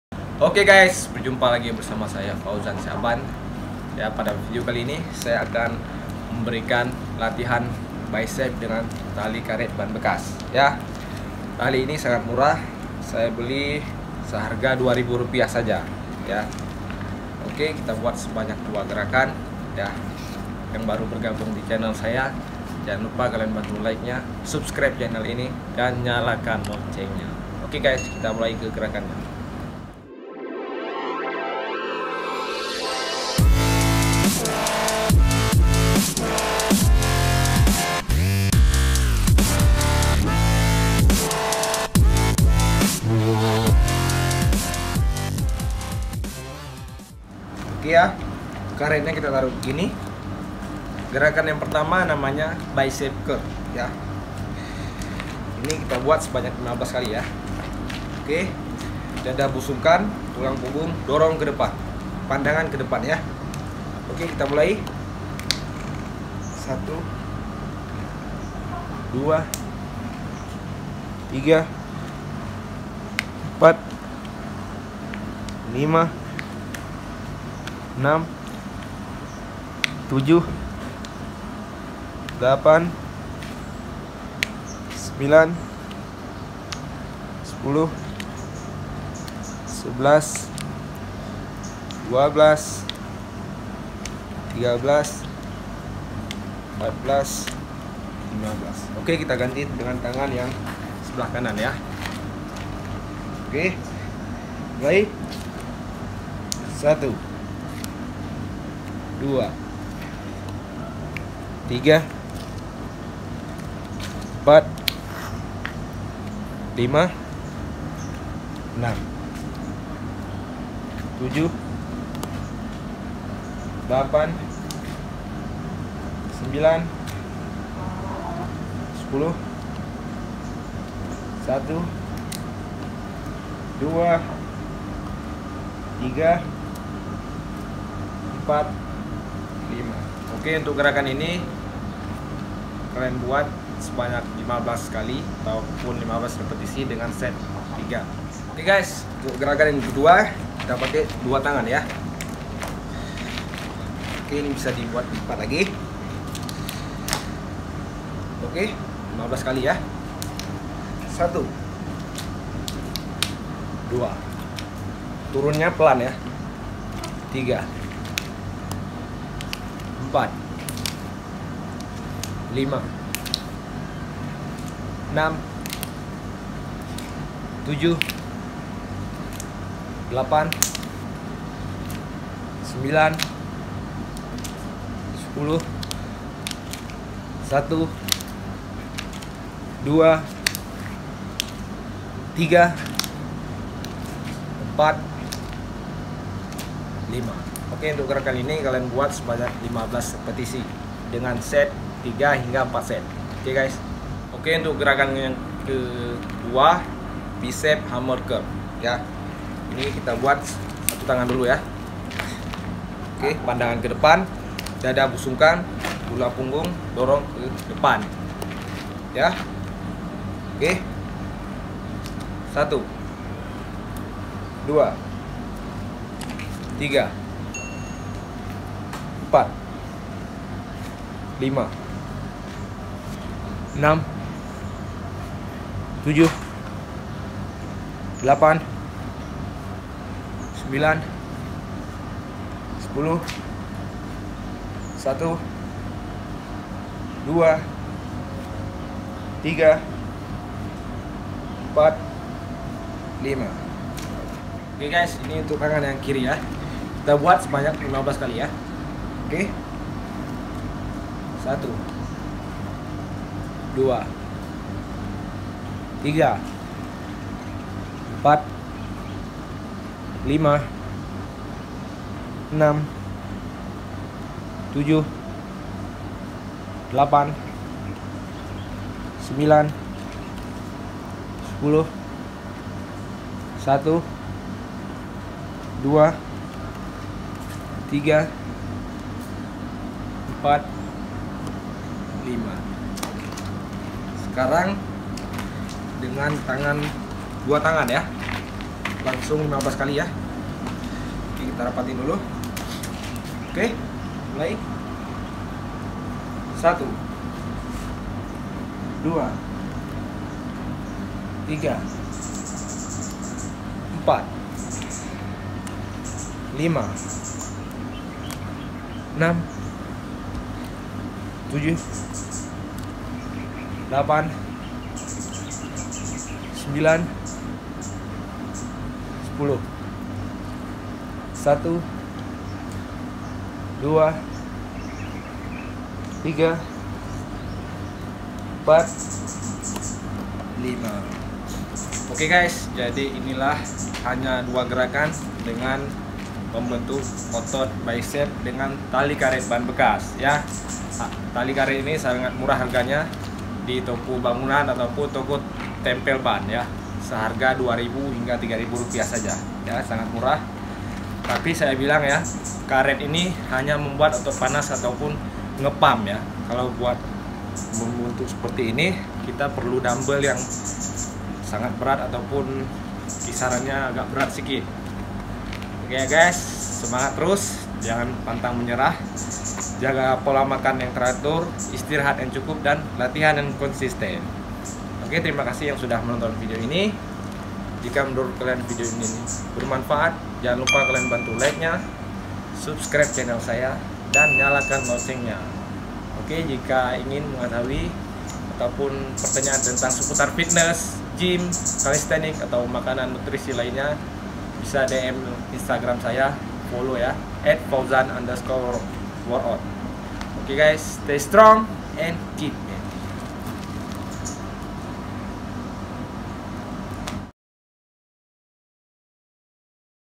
Latihan Otot BICEPS Dirumah tanpa Gym | FAUZAN SYAKBAN
Kali ini saya akan memberikan 2 variasi latihan biceps ?